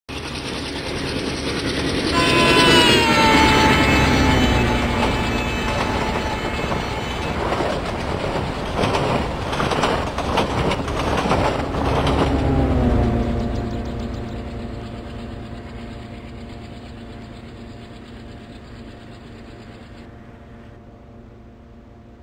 TWIN APU FITTED ERS WDM-3A pulls G.R. to Kochuveli
ERS WDM-3A 16666r + 14048 twins equipped with APU pulls 12201/Mumbai LTT-Kochuveli Garib Rath Express